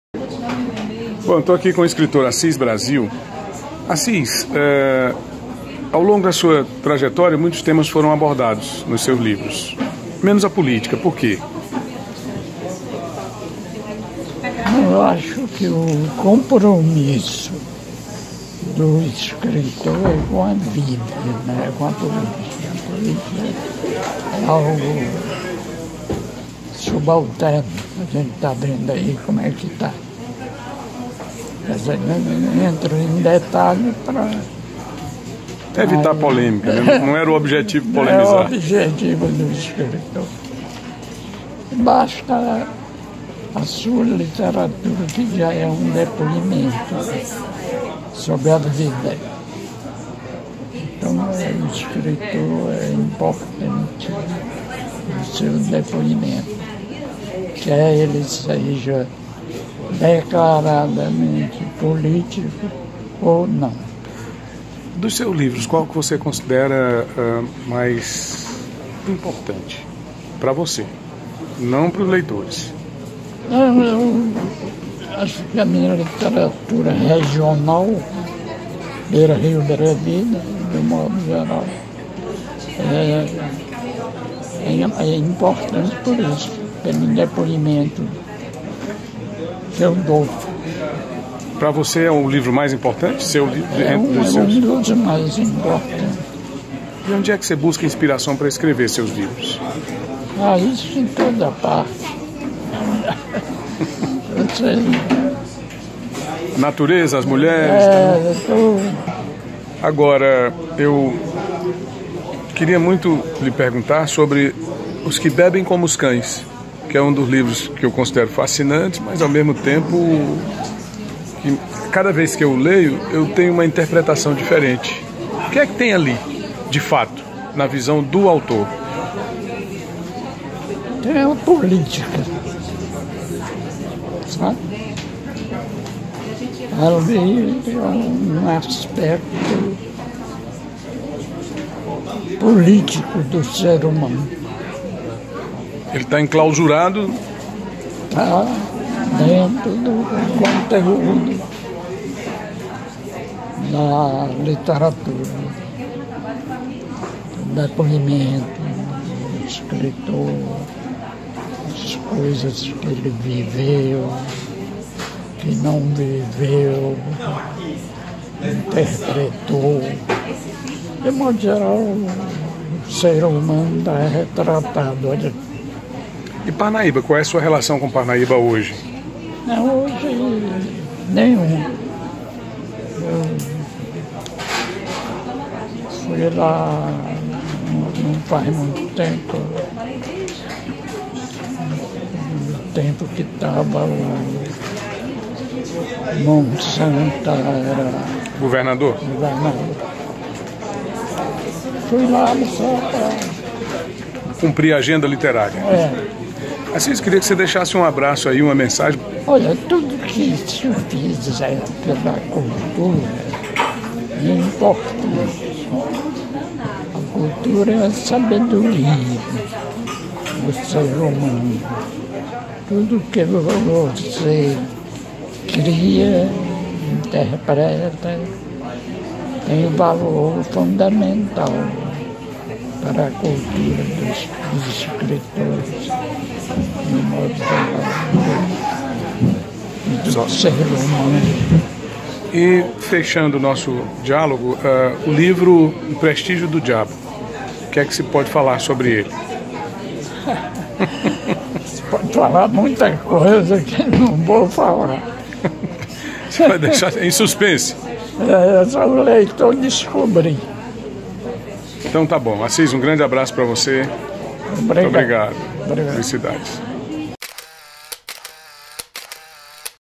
Escritor ASSIS BRASIL fala a TONI RODRIGUES
O escritor Assis Brasil nos concedeu essa entrevista em 2018 por conta do lançamento do seu livro "O Prestígio do Diabo". Com ele falamos sobre temas diversos, como política, literatura, piauiensidade. Assis Brasil disse que o livro "Beira Rio Beira Vida" é importante para a literatura regional. "Um dos (meus livros) mais importantes." O escritor disse que busca inspiração em toda parte. Ele falou também sobre o emblemático "Os que bebem como os cães". Esse é um livro que trata sobre o aspecto político do ser humano. Assis Brasil faleceu neste domingo 28 de novembro aos 93 anos de idade, mantendo a lucidez impressionante que sempre o caracterizou. O escritor falou ainda sobre sua relação com Parnaíba, cidade natal da qual se ausentou desde o livro "Beira Rio Beira Vida".